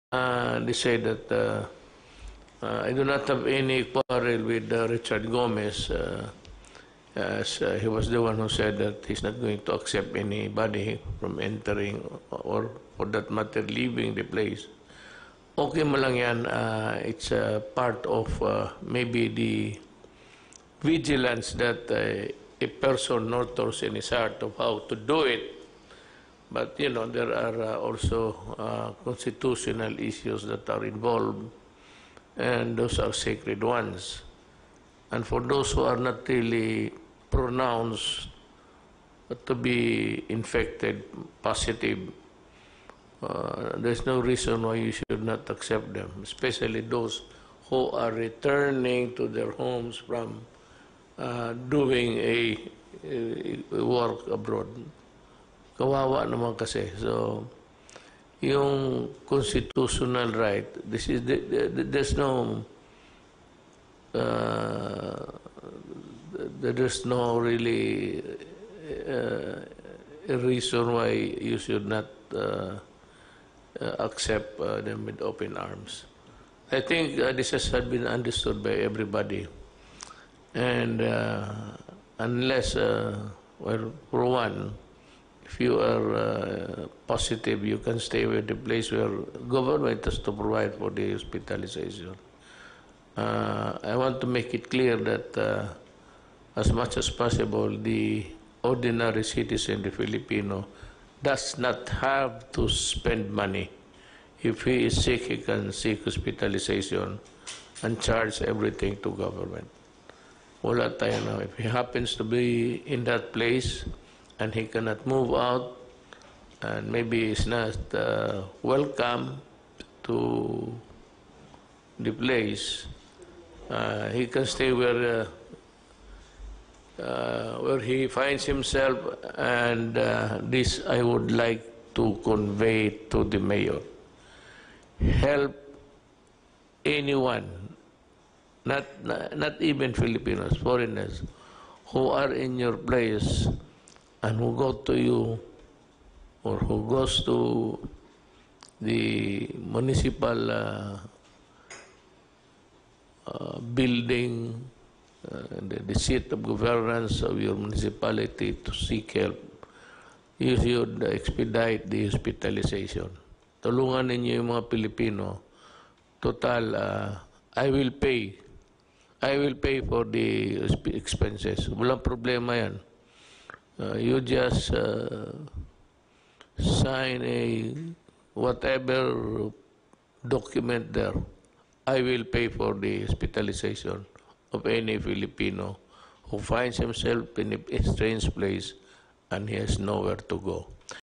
FACT CHECK: Duterte on Richard Gomez "not accepting" returnees in Ormoc
More videos on Rappler On Thursday, May 28, President Rodrigo Duterte said in a late night address that Ormoc City Mayor Richard Gomez said he would not accept anyone returning from the city to enter the city. But Gomez only complained about the national government’s lack of coordination and did not say he was going to refuse the returning residents Follow Rappler for the latest news in the Philippines and around the world.